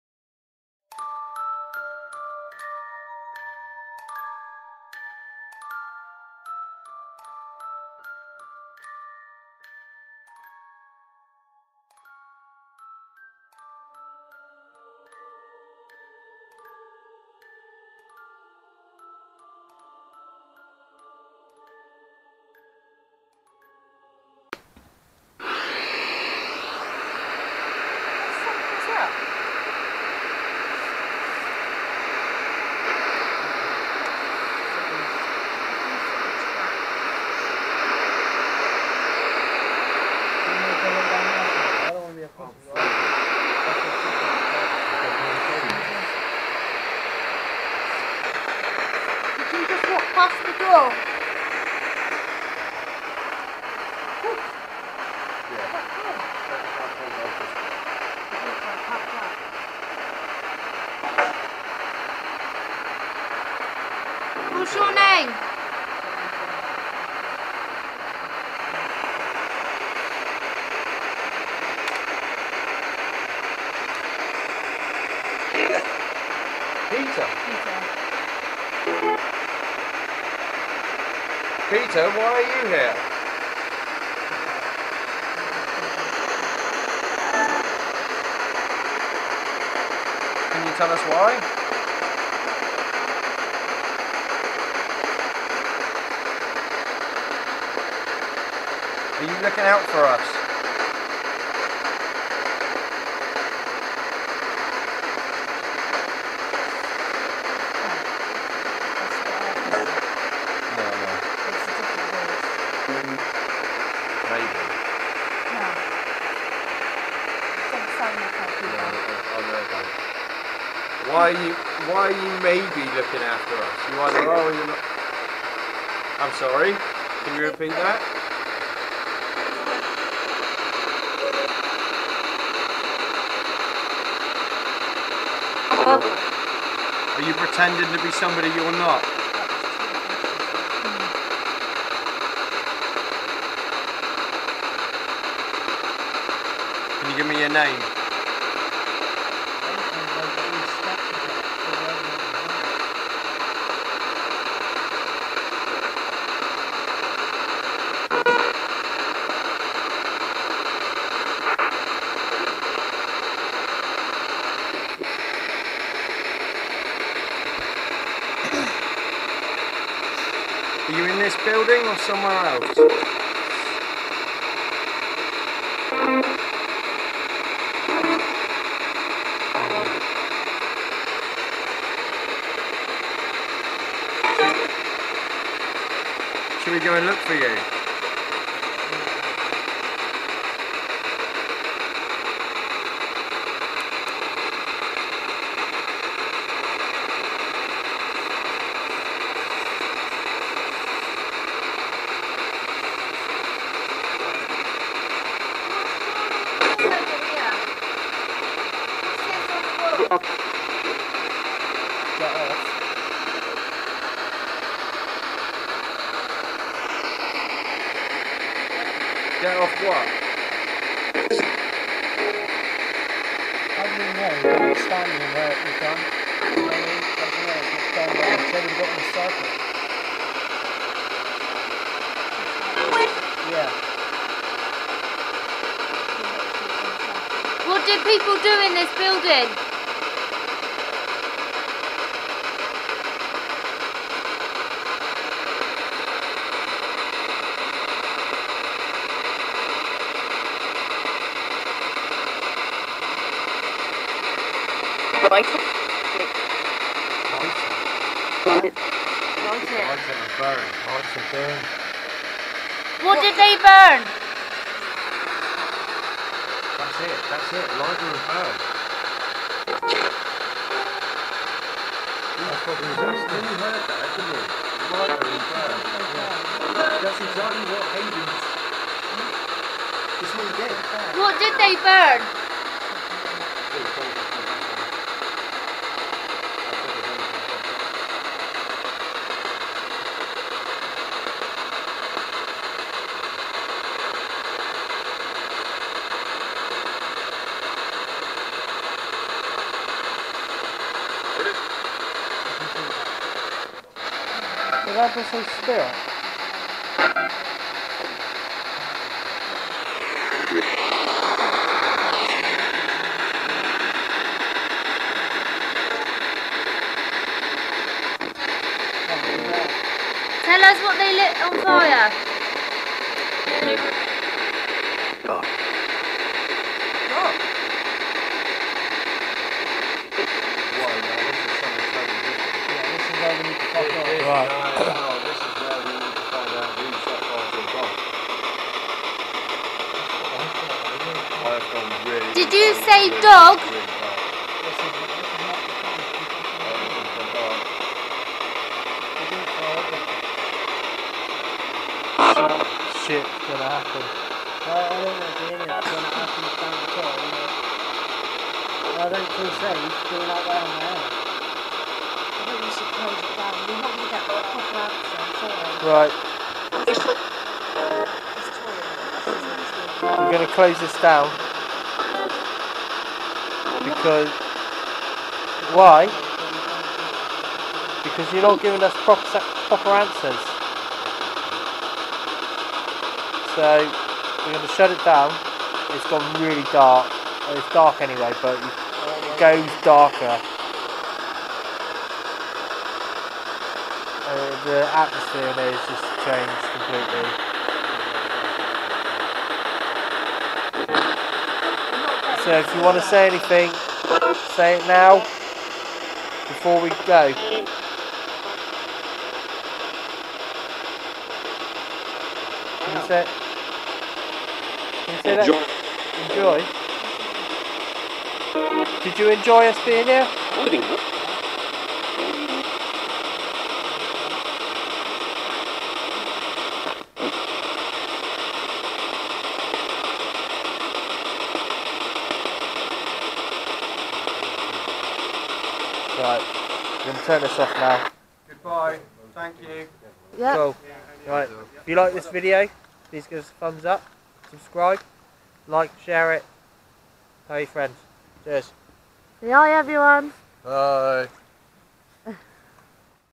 kennel vale part 3
this is part 3 of are investigation at kennel vale please use headphones when watching this video